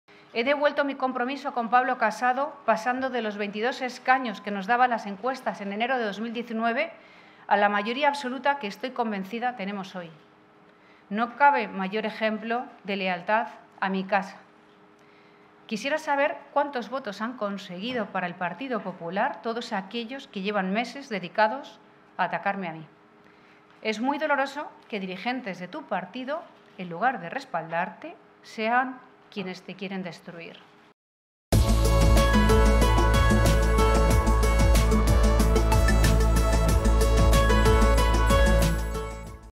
Ayuso: "Cuántos votos han conseguido los que llevan meses dedicados a atacarme a mí"
"No puede haber nada más grave que acusar a alguien de la propia casa, con responsabilidades de gobierno, de corrupción. Y hacerlo sin pruebas".